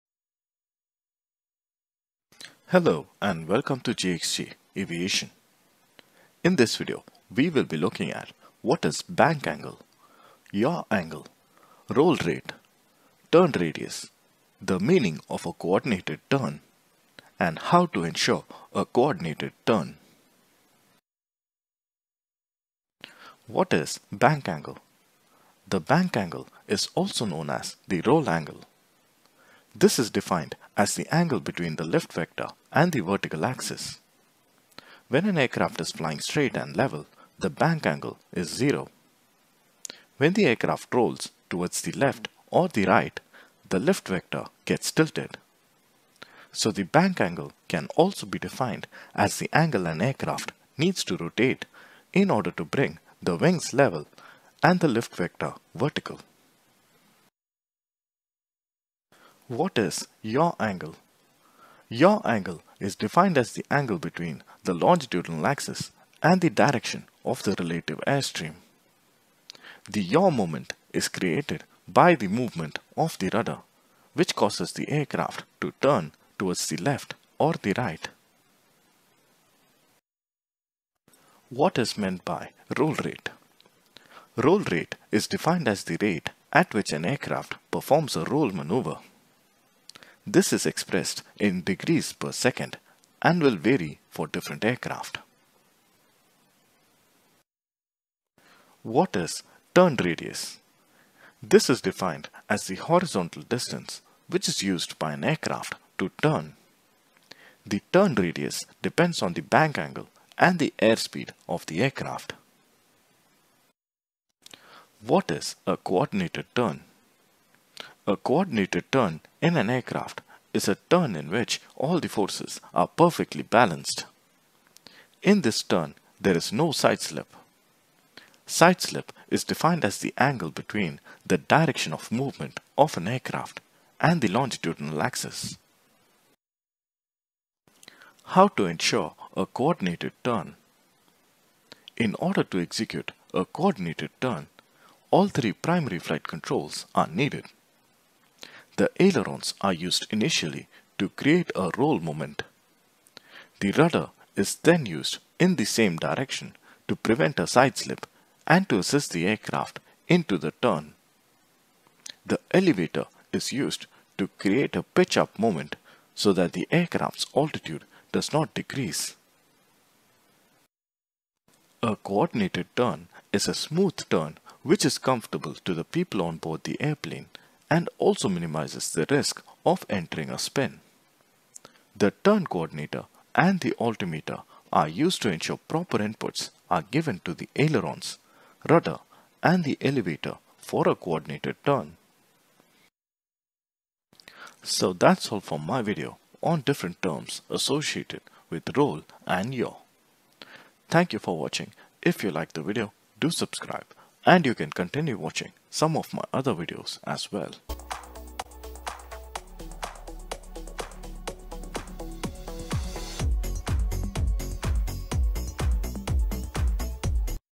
What are Bank, Yaw Angles; Roll Rate; Turn Radius; Coordinated Turn| Maintaining a Coordinated Turn
Hi. in this video we look at some definitions of a turning flight such as bank angle, yaw angle, roll rate, turn radius; coordinated turn and how to maintain a coordinated turn.

We see the advantages of maintaining a coordinated turn and how all the primary flight controls are used to maintain a coordinated turn.